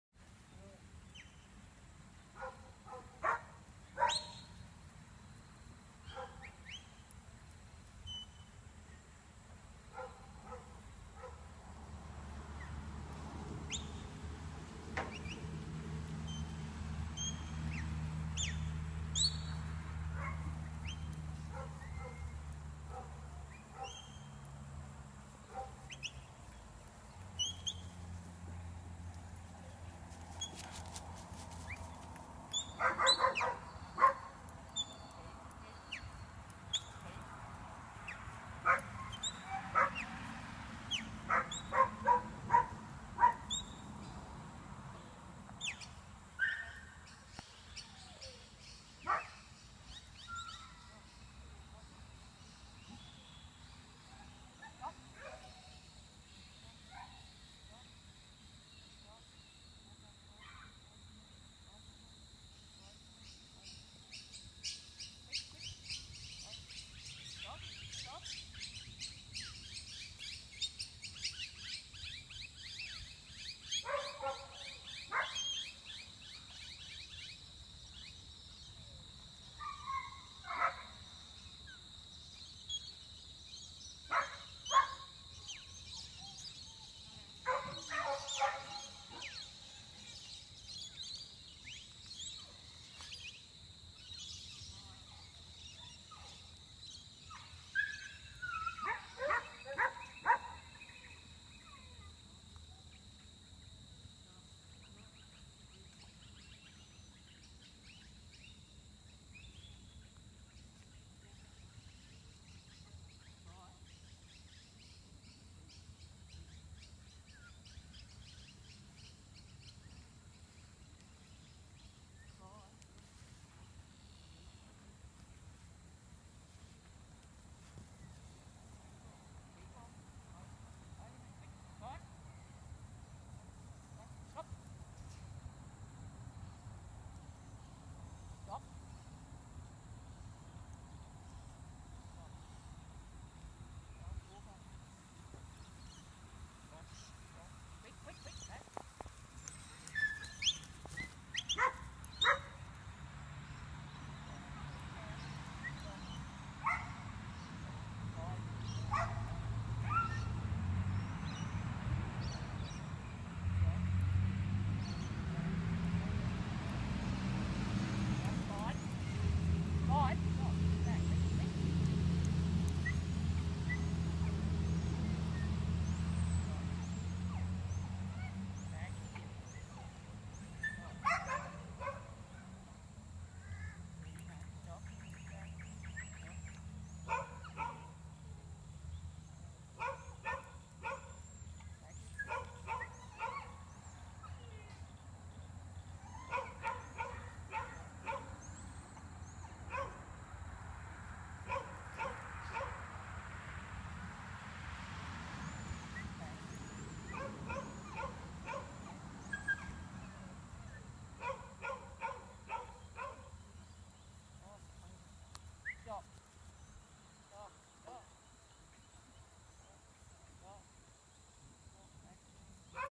Barru Spiderman 2015
Barru Spiderman (Karrawarra Boozer/Karrawarra Lisa) shows the lovely control that he has accepted from Susie Goodyear. While his outside work is very natural he has also taken home a number of ribbons from yard dog trials in his first year of trialling.